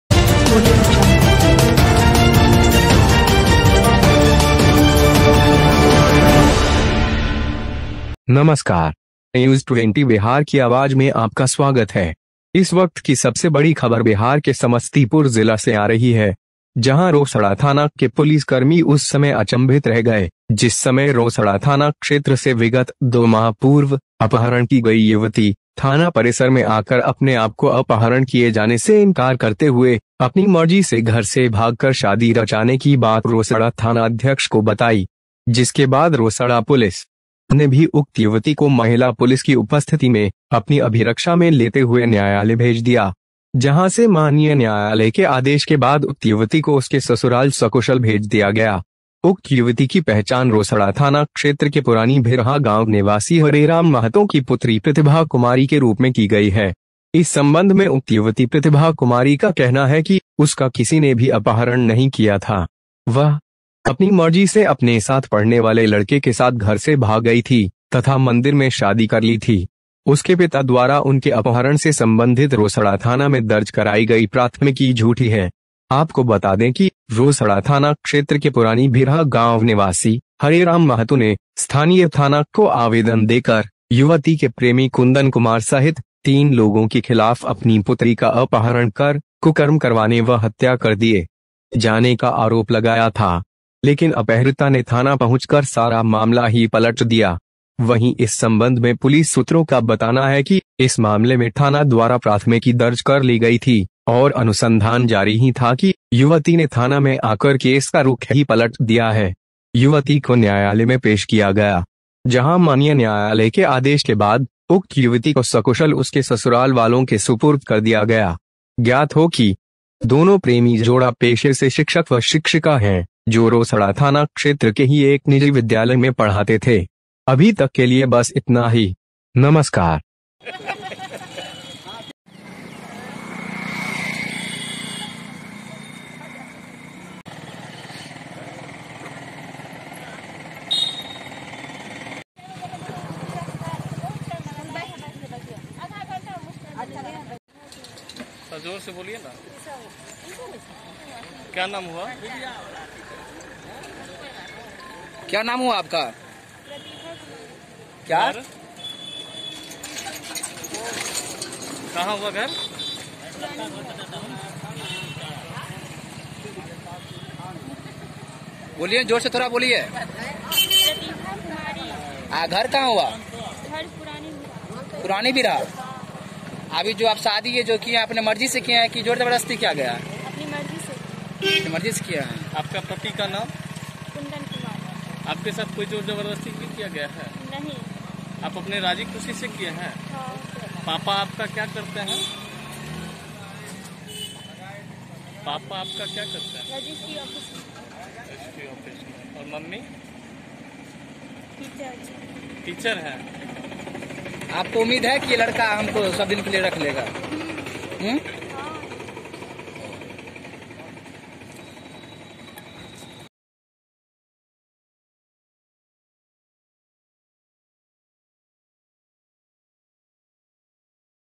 Samastipur:अपहृत लड़की पहुंची थाने कहा अपहरण नही हुआ, प्रेमी के साथ भागी थी।@NEWS20BIHARKIAWAZ।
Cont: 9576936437